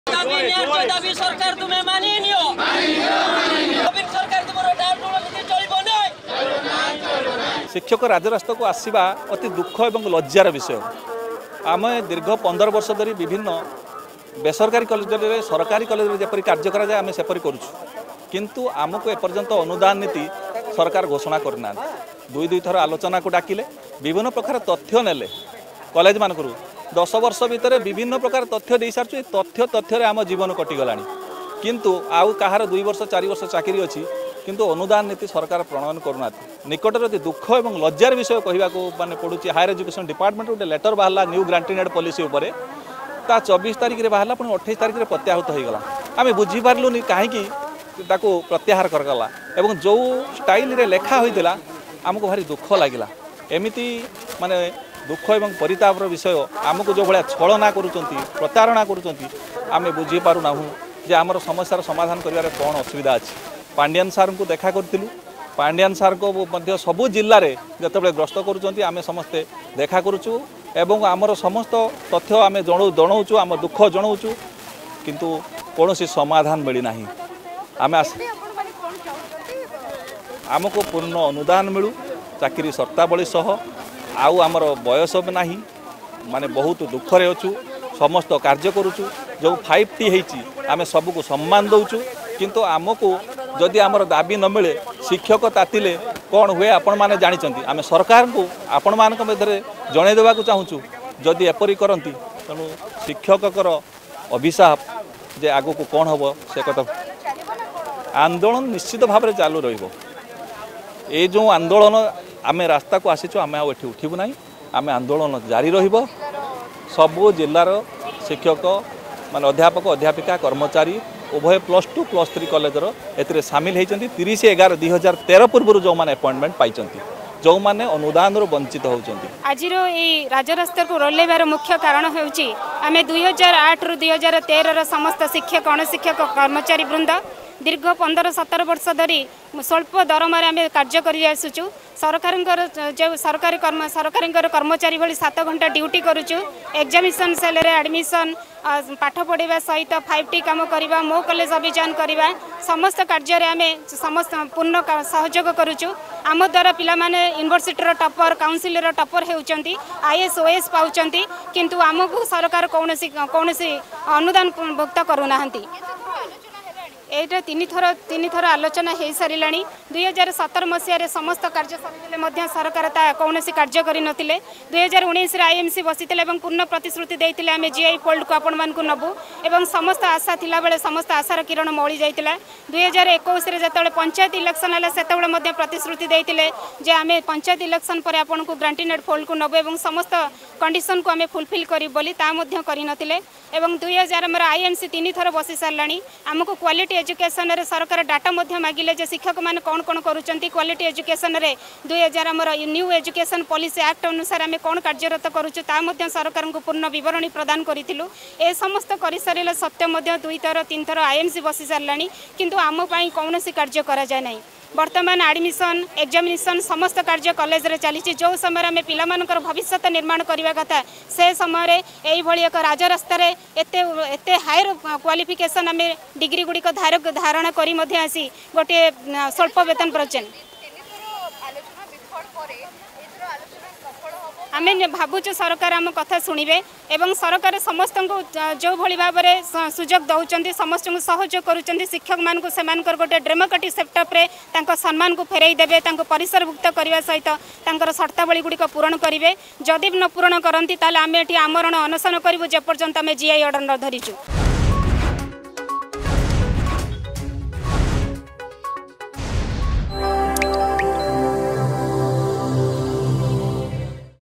Non Govt Aided & Unaided Teaching & Non Teaching Staff Stage protest Over Several Issue
Non Govt Aided & Unaided Teaching & Non Teaching Staff Stage protest Over Several Issue In Bhubaneswar